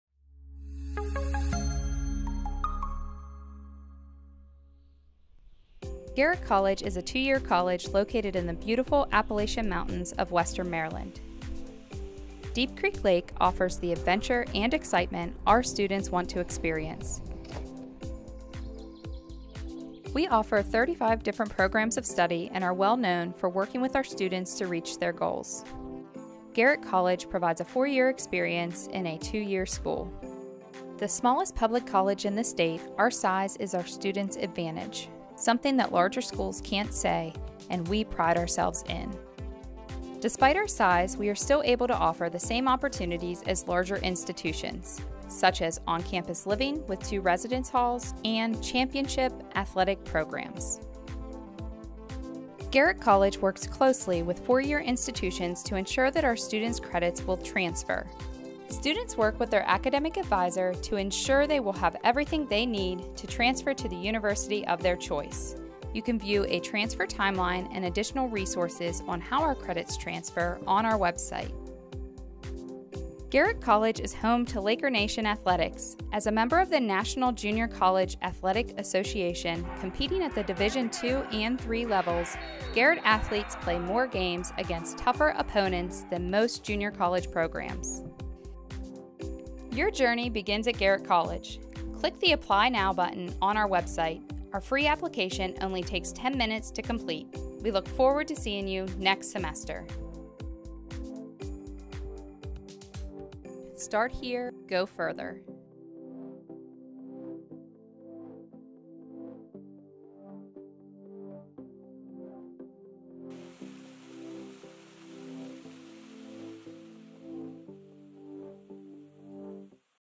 Garrett College - Start Here, Go Further
Begin your educational journey at Garrett College in western Maryland. We offer a wide variety of academic programs of study, on-campus living options, athletic programs, small class sizes, and academic support services designed to help you reach your goals and succeed.